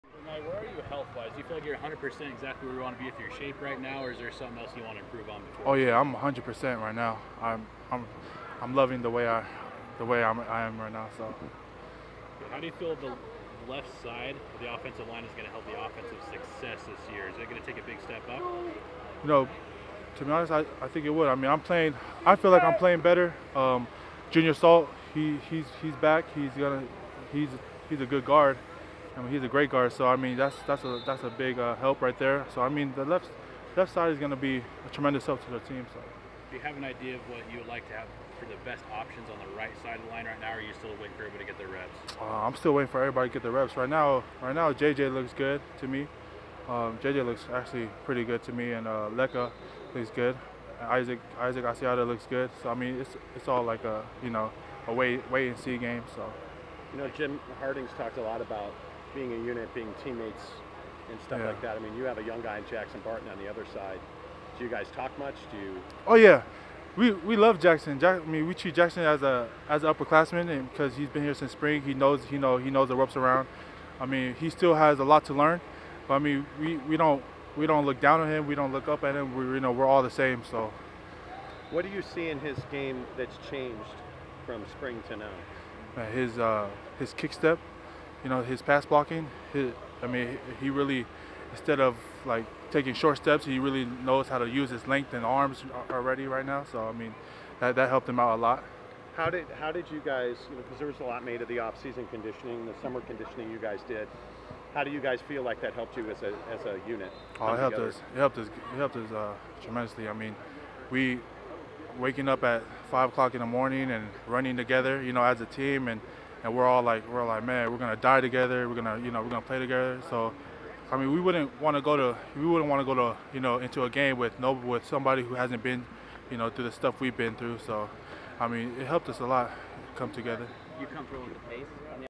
Jeremiah Poutasi
Poutasi says Jackson Barton has earned the respect of his fellow linemen!